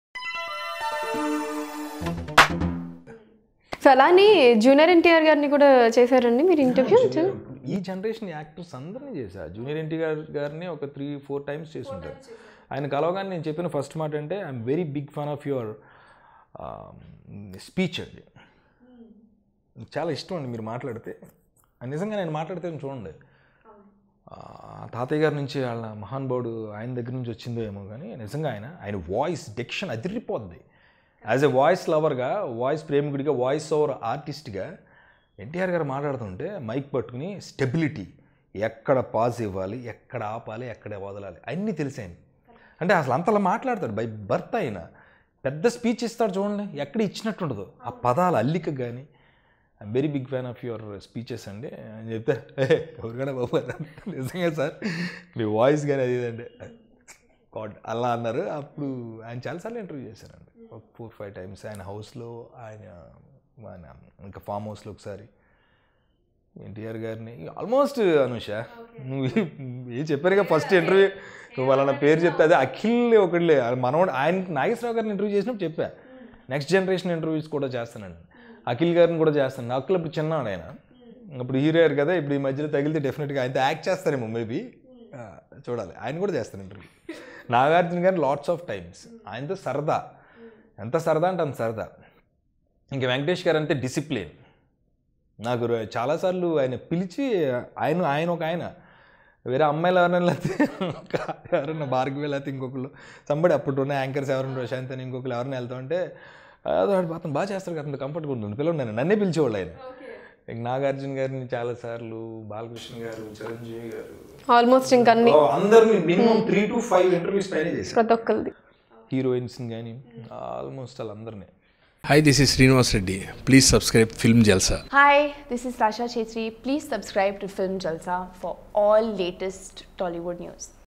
Actor Gemini Suresh On Jr Ntr Attitude | Exclusive Interview | Film Jalsa
GeminiSuresh On JrNtr Attitude, Exclusive Interview ,Film Jalsa